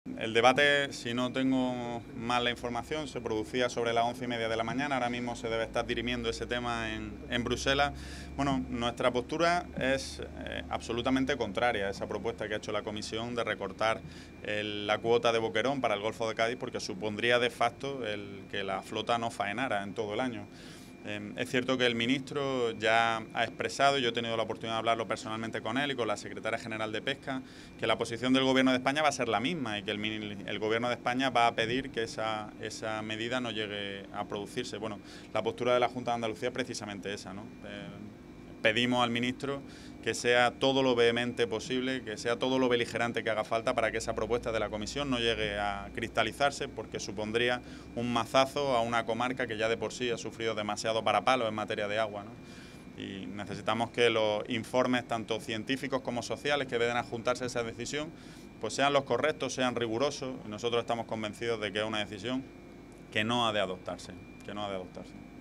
Junta pide al Gobierno que sea "beligerante" contra la propuesta sobre cuotas de anchoa
El consejero de Agricultura, Pesca, Agua y Desarrollo Rural, Ramón Fernández-Pacheco, ha instado al Gobierno a que sea "todo lo vehemente posible, que sea todo lo beligerante que haga falta" para que no avance la propuesta de la Comisión Europea para reducir las cuotas de anchoa para 2024 y 2025, una medida que puede afectar a la pesca en el golfo de Cádiz.